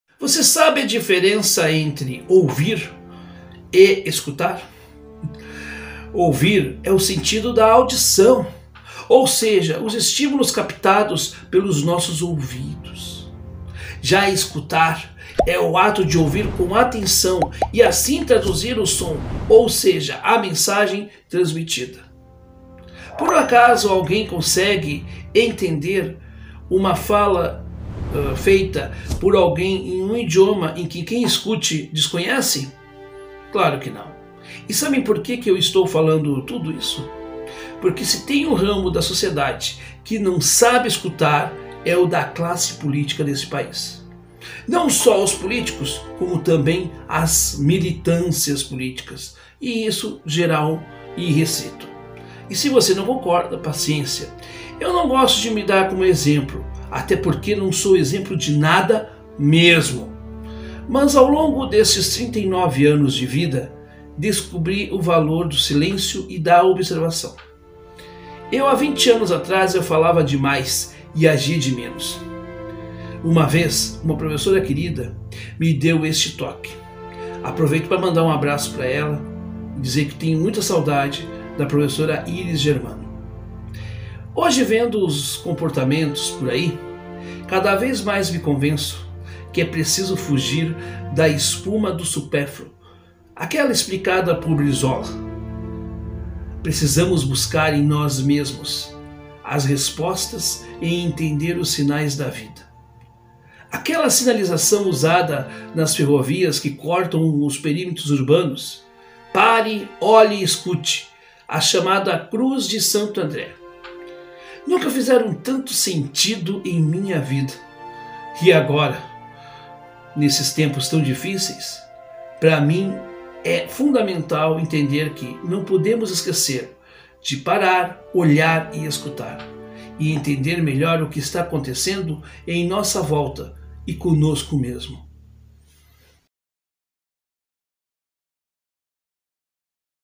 Você sabe a diferença?
Você sabe a diferença entre OUVIR e ESCUTAR?

OUVIR é o sentido da audição, ou seja, os estímulos captados pelos nossos ouvidos.

Já ESCUTAR é o ato de ouvir com atenção e assim "traduzir" o som, ou seja, a mensagem transmitida.
Alguém consegue por um acaso decodificar alguma mensagem emitida por alguém que fale em um idioma que quem escute desconhece?
Sabem porque eu estou falando tudo isso? Porque se tem um ramo da sociedade que não sabe ESCUTAR é o da classe política deste país! Você não concorda?

Não gosto de me usar como exemplo de nada, até porque não sou exemplo de nada mesmo.
Mas eu ao longo destes 39 anos descobri o valor do silêncio e da observação. Eu há 20 anos atrás eu falava demais e agia de menos. Uma vez, uma querida professora me deu este toque, aproveito pra mandar um abraço para professora Iris Germano.

Hoje vendo os comportamentos, cada vez mais me convenço que é preciso fugir da espuma da surpefluo explicada por Brizola!

Precisamos buscar em nós mesmos as respostas e entender os sinais da vida!

Aquela sinalização usada nas ferrovias que cortam o perímetro urbano  PARE, OLHE E ESCUTE, a cruz de Santo André nunca fizeram tanto sentido em minha vida, como agora!

Não esqueça PARE, OLHE E ESCUTE e entenda melhor o que está acontecendo em tua volta e contigo!